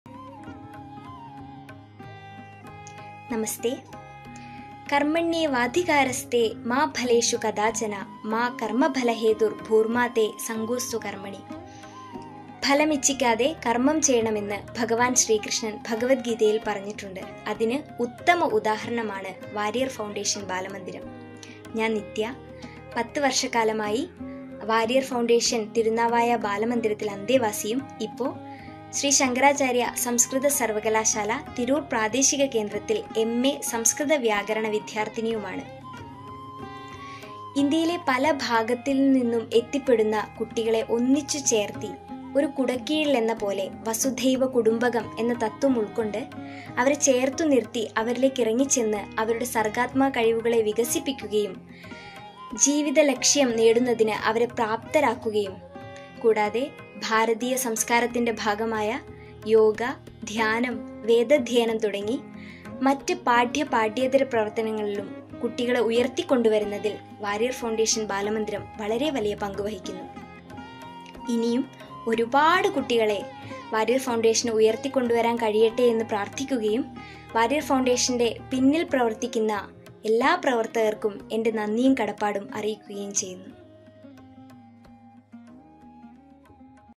Pratiroop - Dance for a Cause | Warrier Foundation | Nitya - Student, WF's Thirunavaya Balamandiram
Click the below link to donate to Warrier Foundation's cause of providing infrastructure and resources to 300 underprivileged children

Nitya, came to Warrier Foundation's Balamandir at the age of 12. She was fascinated by the Sanskrit language and with the support and backing of Warrier Foundation, Nitya is currently pursing her M.A in Sanskrit. This enthusiastic younger, who is also a Yoga Master, believes that the support she received from Warrier Founfation has helped her reach where she is today. She aims to complete her B.Ed and work as a teacher. But her long term goal is to be an IAS officer. Warrier Foundation is proud to be a part of the growth and success story of Nitya and several others like her.